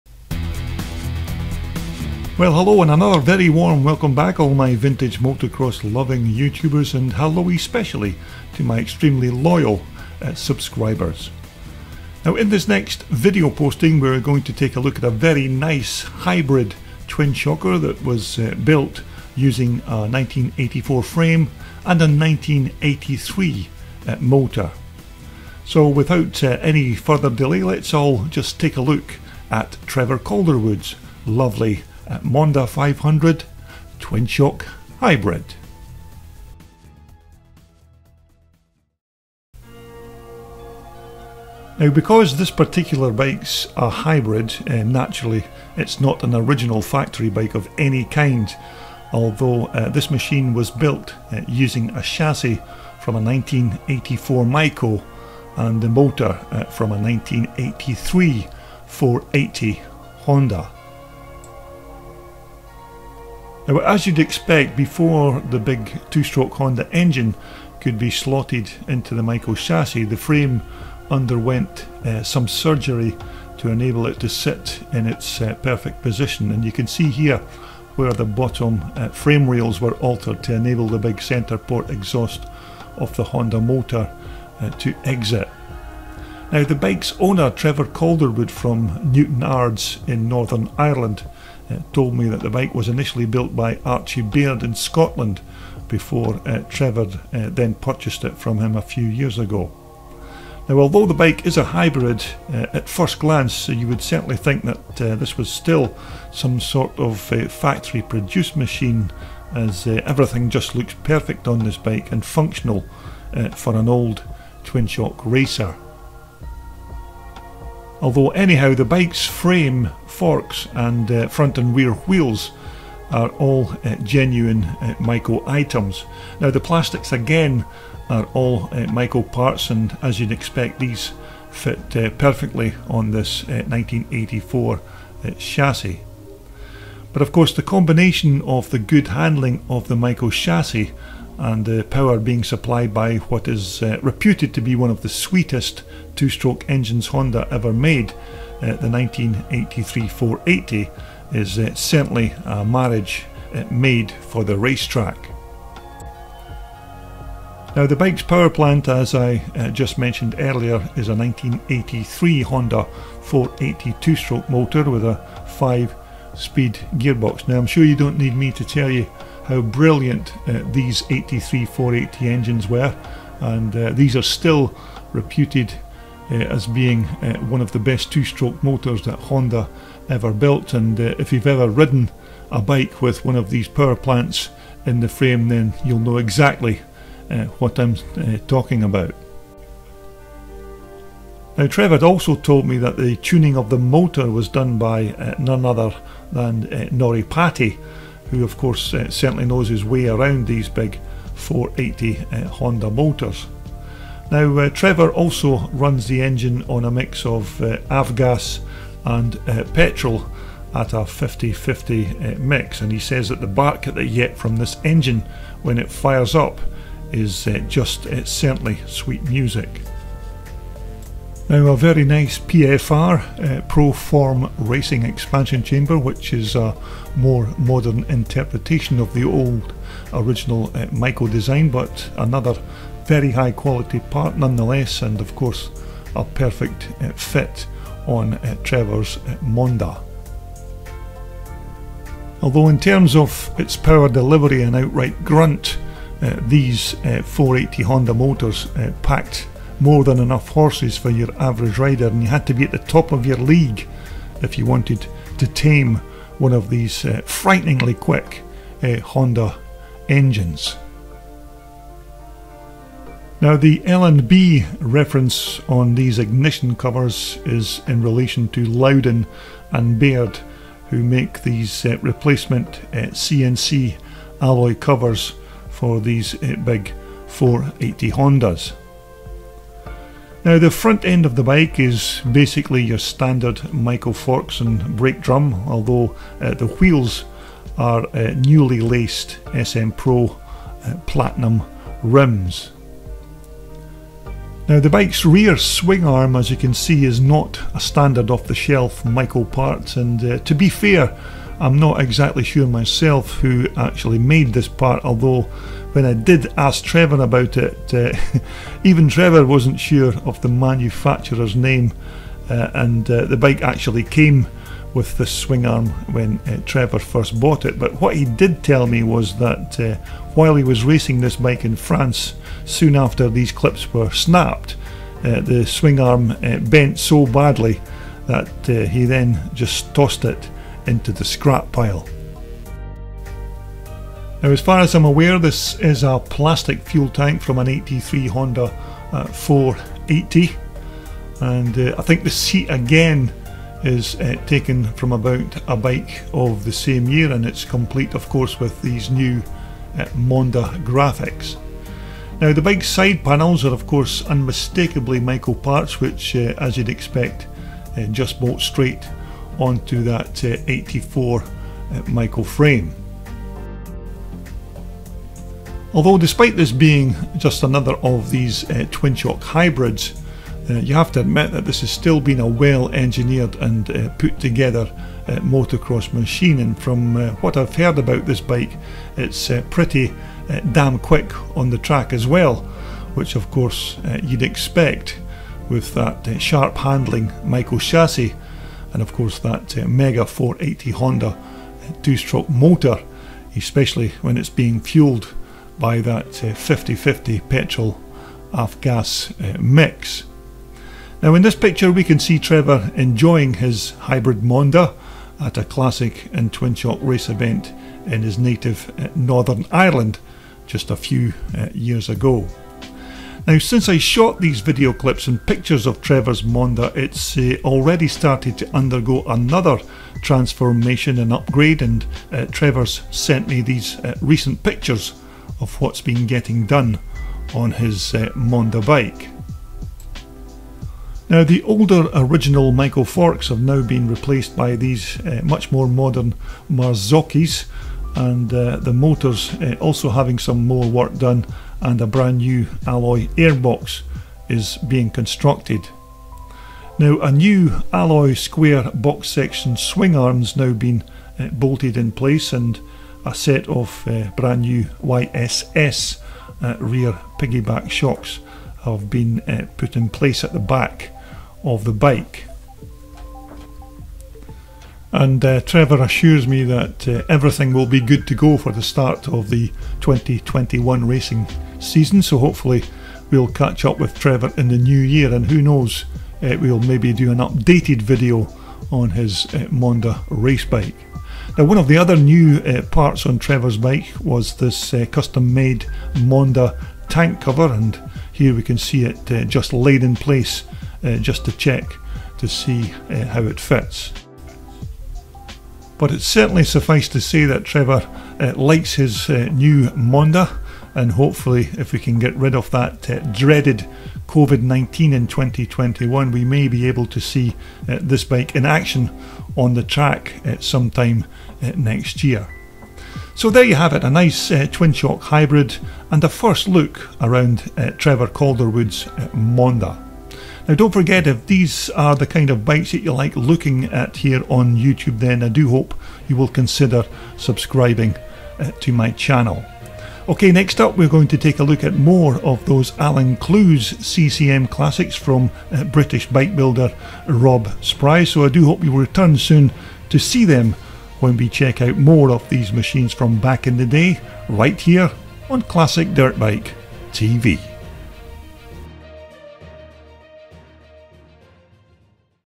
Classic Dirt Bikes "Monda 480 Hybrid Twinshocker"
A quick look around Irishman Trevor Calderwood's "Monda" Twinshock race bike. This machine is built using a 1984 Maico chassis and a 1983 Honda CR480 engine. These types of hybrid concoction's are now very popular in Twinshock motocross and this version of Trevor's is another very nice machine.